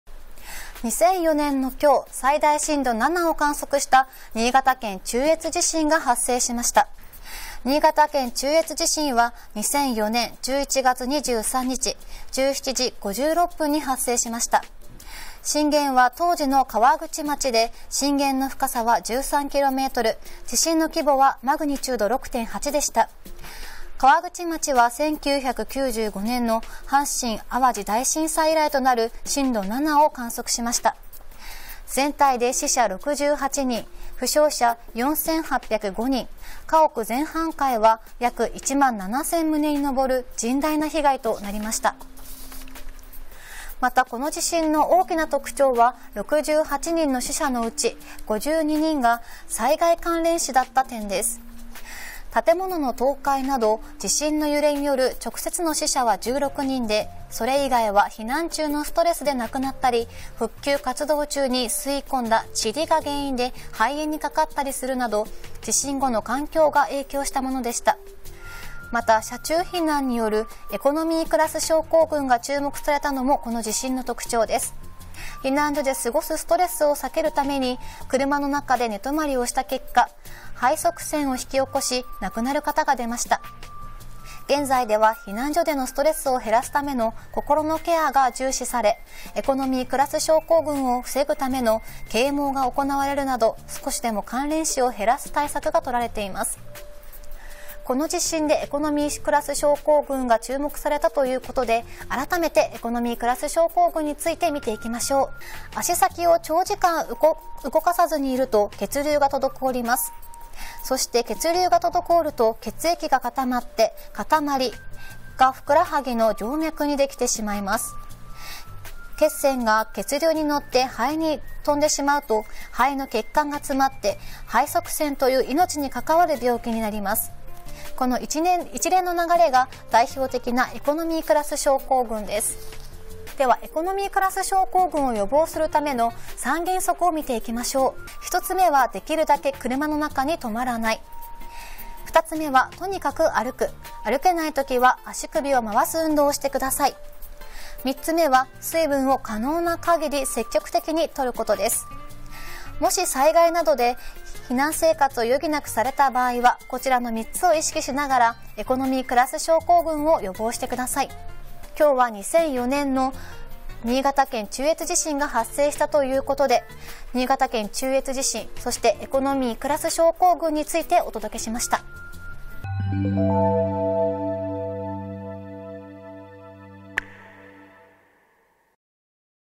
2004年の今日 新潟中越地震発生 教訓と対策
2004年の今日、最大震度7を観測した、「新潟県中越地震」が発生しました。その時の状況やそこからの教訓・対策を確認していきます。